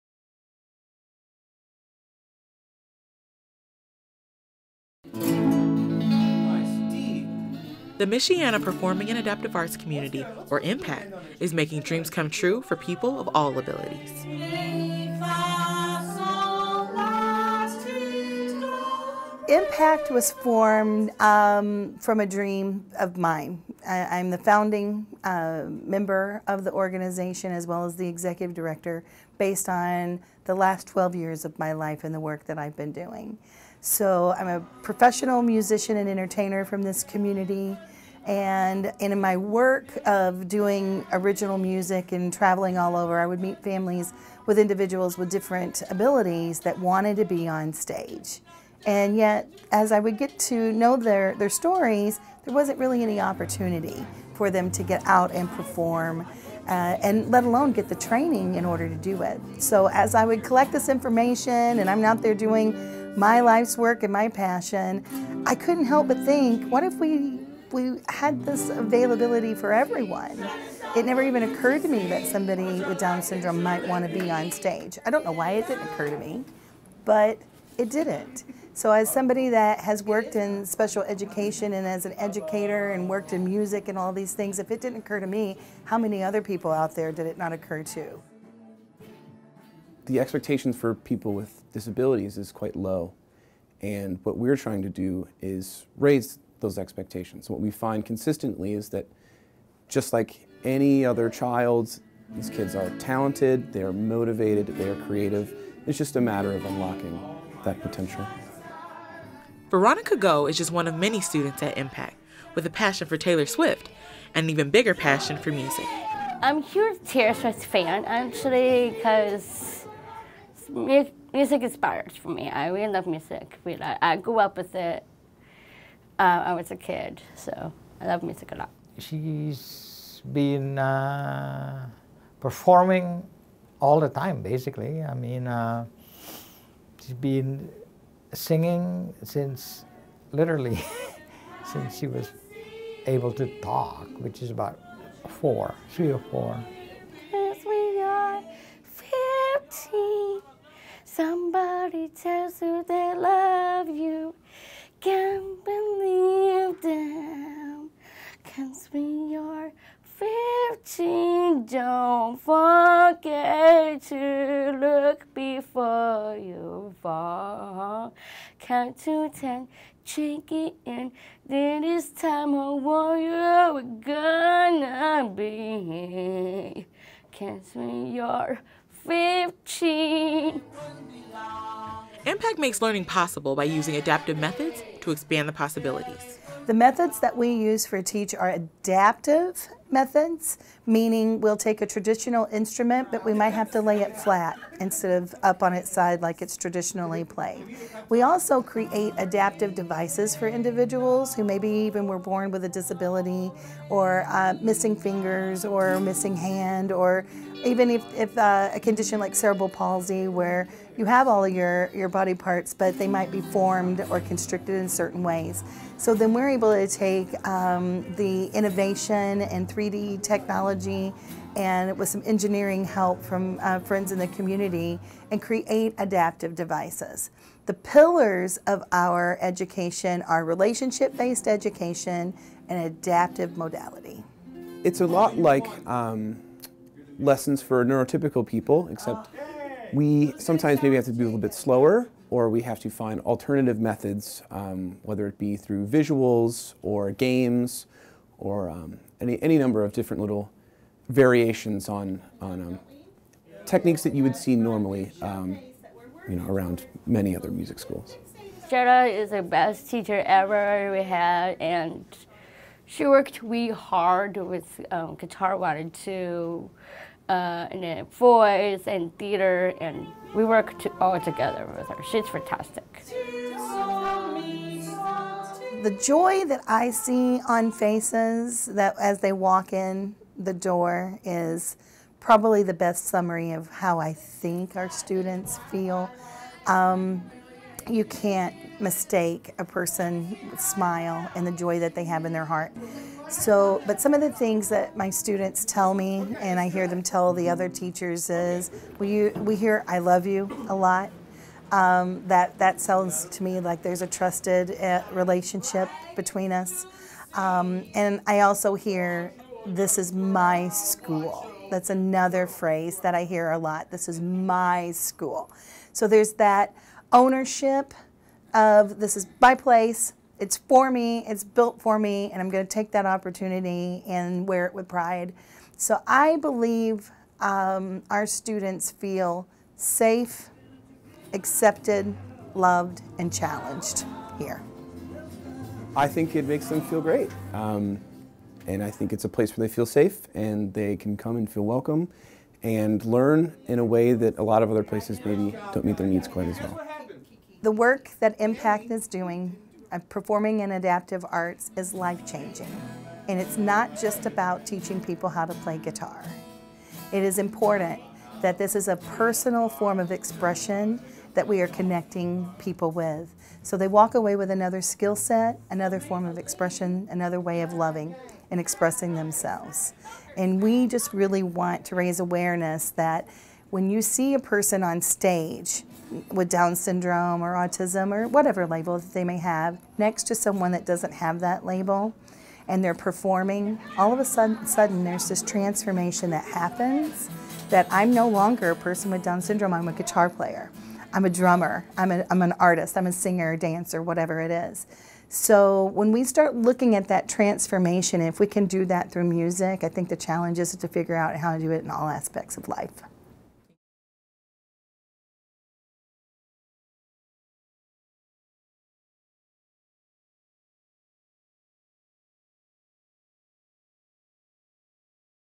Education Counts Michiana - Michiana Performing and Adaptive Arts Community
The Michiana Performing and Adaptive Arts Community is giving people of all abilities the chance to express themselves through music and using alternative methods to teach students with special needs. These methods include alternative teaching styles and the use of adaptive devices.